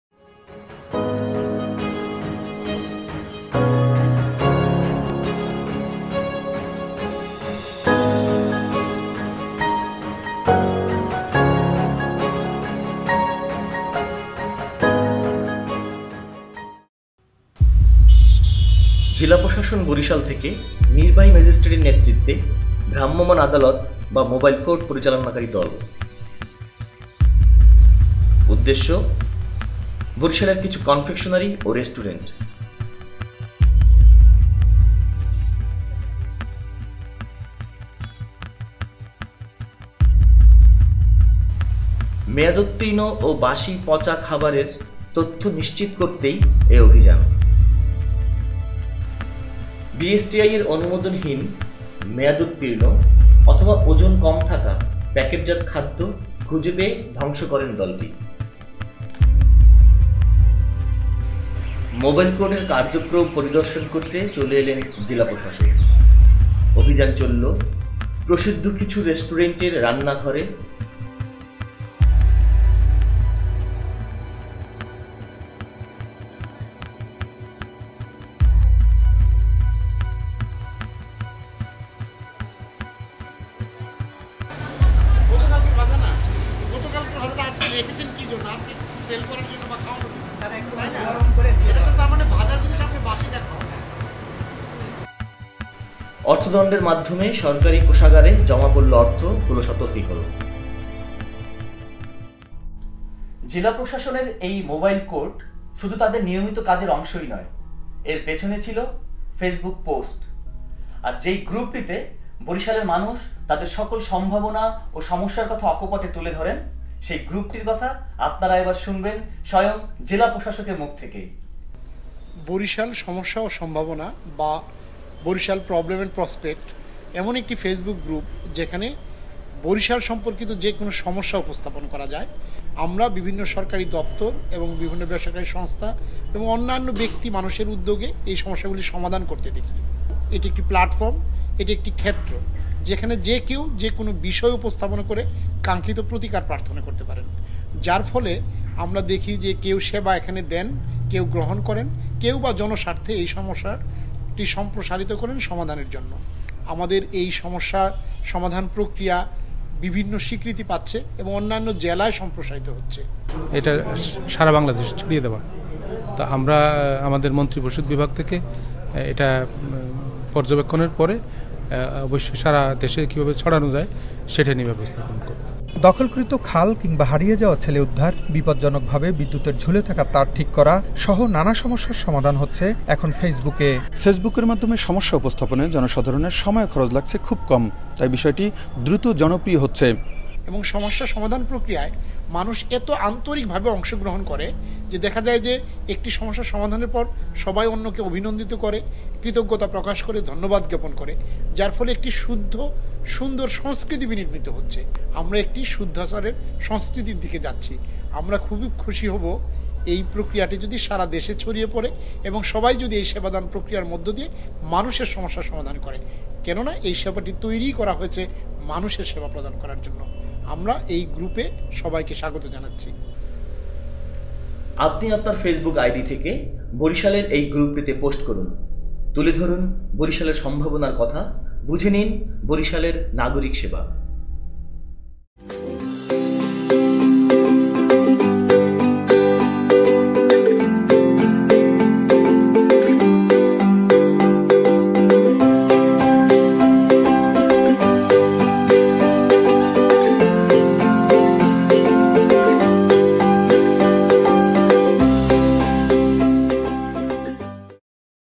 Barisal problem & prospect
বরিশাইল্যা রেনেসাঁ :
Documentary on Facebook group "Barisal - Problem & Prospect বরিশাল - সমস্যা ও সম্ভাবনা"